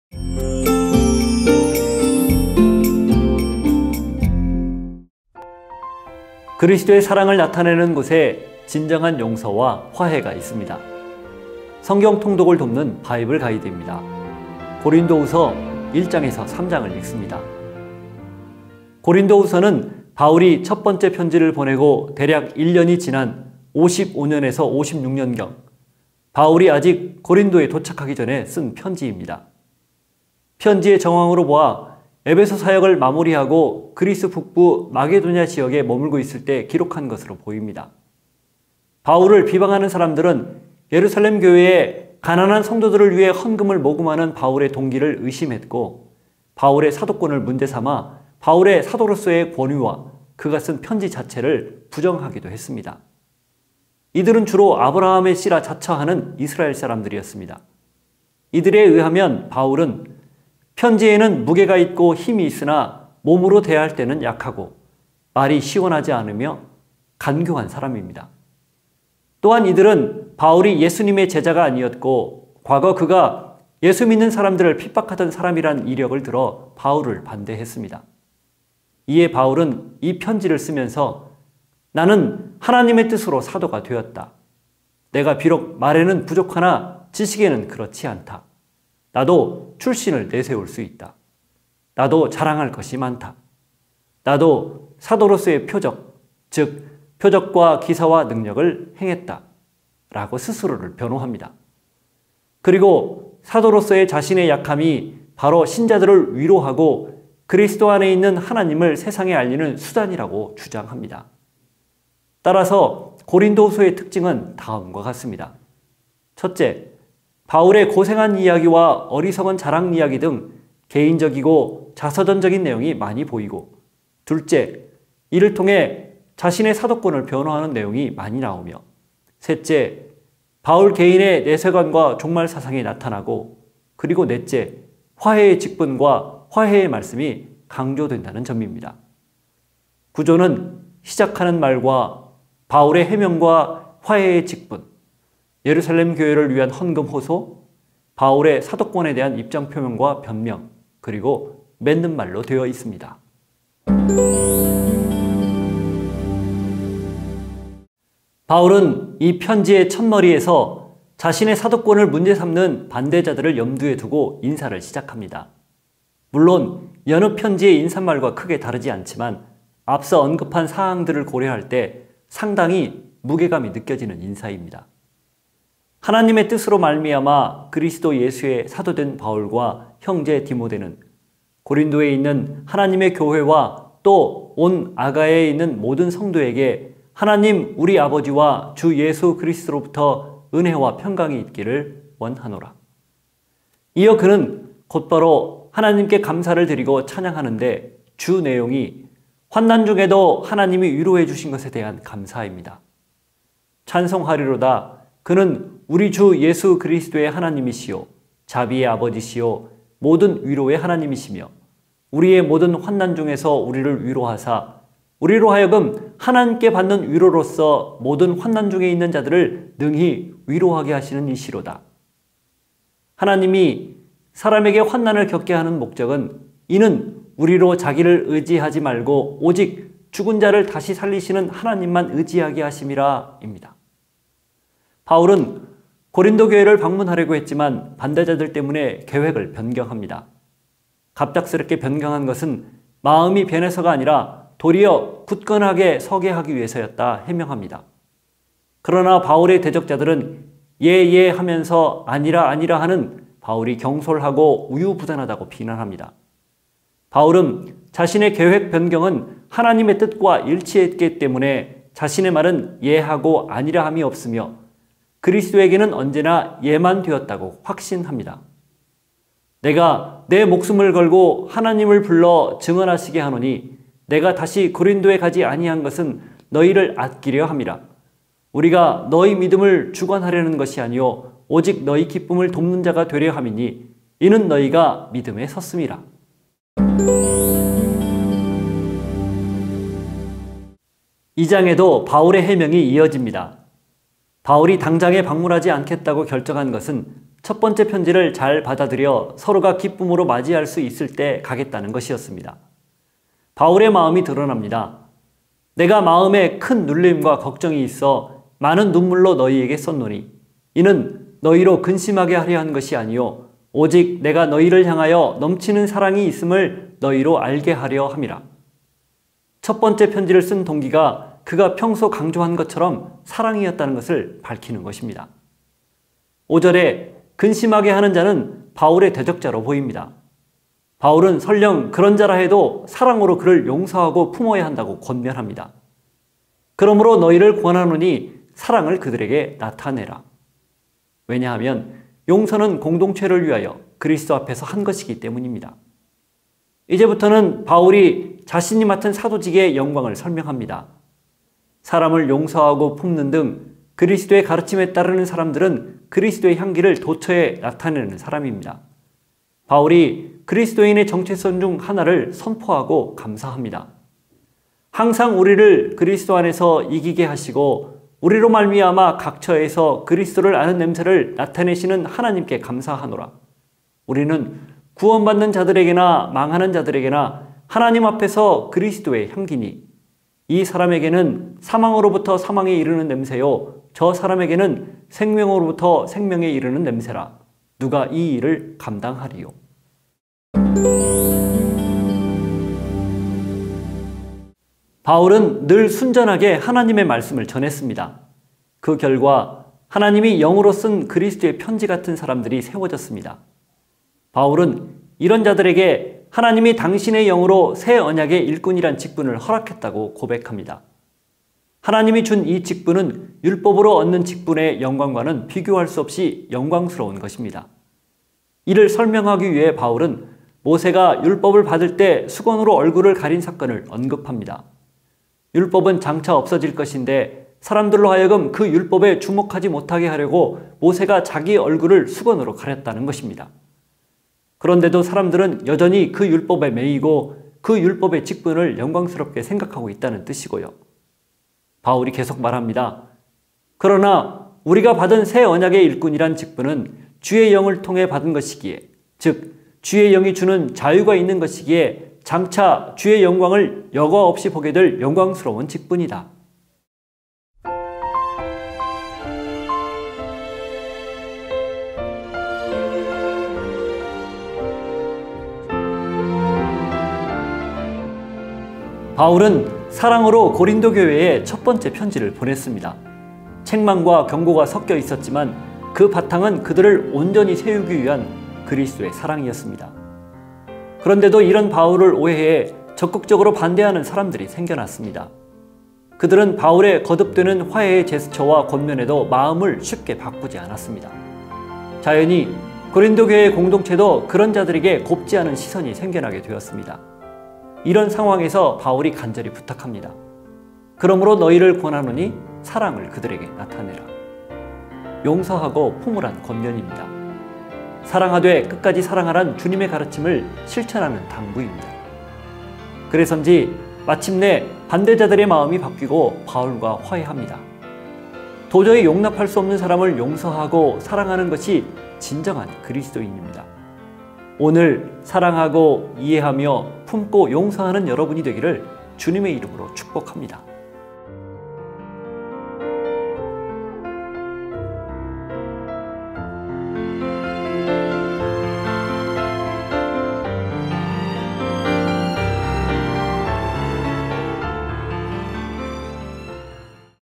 [성경해설]고린도후서 1장 2장 3장 | 바이블가이드
[1년 성경 통독을 위한 7분 가이드]
1장: 3)찬미와 감사, 12)바울의 방문 계획 변경
2장: 5)잘못한 자를 용서하라, 12)그리스도를 아는 냄새
3장: 그리스도의 편지